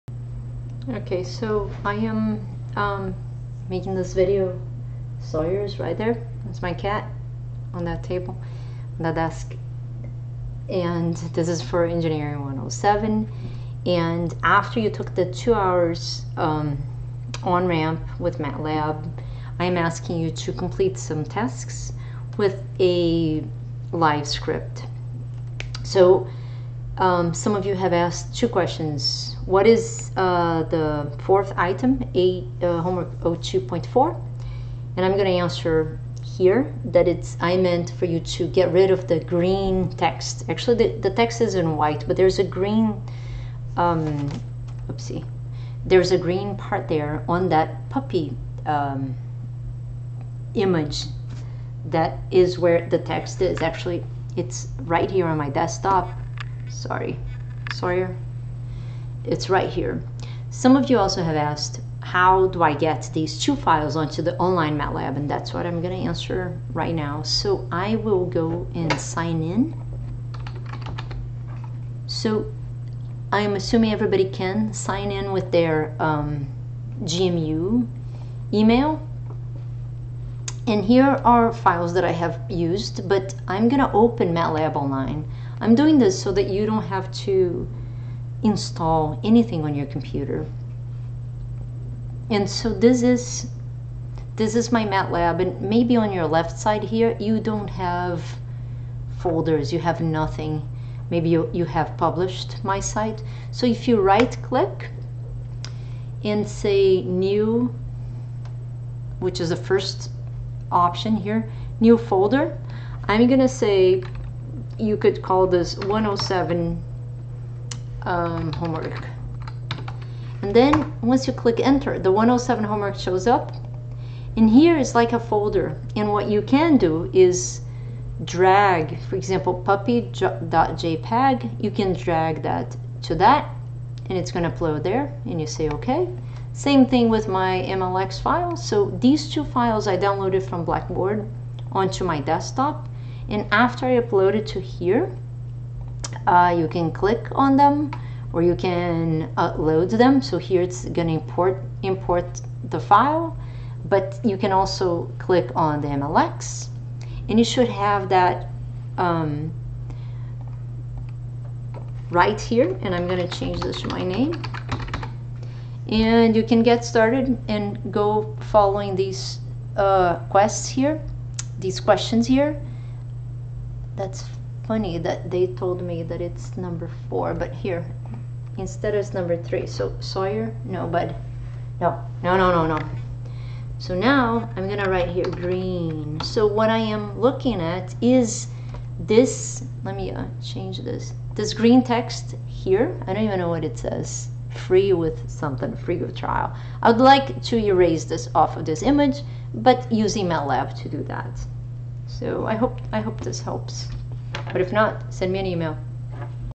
Online Matlab - how to get files to your drive
ENGR107 - how to understand homework 02 - how to get those two blackboard files to the online drive.